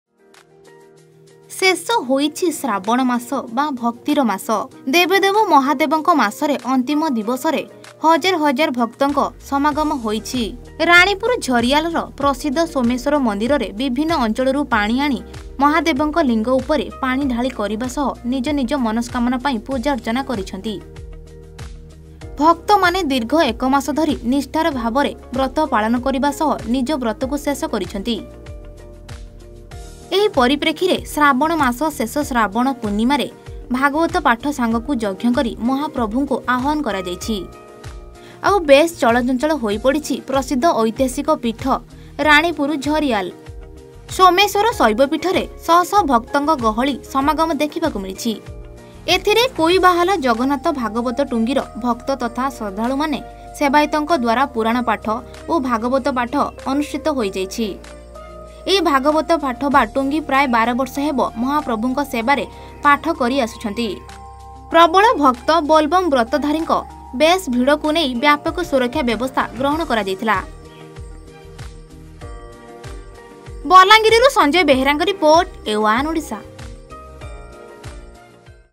ଶେଷ ଶ୍ରାବଣ ପୂର୍ଣ୍ଣିମାରେ ପୂଜାର୍ଚ୍ଚନା ତଥା ଭାଗବତ ଟୁଙ୍ଗୀ ପୁରାଣ ପାଠ | A1 ODISHA
ଚଉଷଠି ଯୋଗିନୀ ମନ୍ଦିର ରାଣୀପୁର୍ ଝରିୟାଲରେ ଶେଷ ଶ୍ରାବଣ ପୂର୍ଣ୍ଣିମାରେ ପୂଜାର୍ଚ୍ଚନା ତଥା ଭାଗବତ ଟୁଙ୍ଗୀ ପୁରାଣ ପାଠ l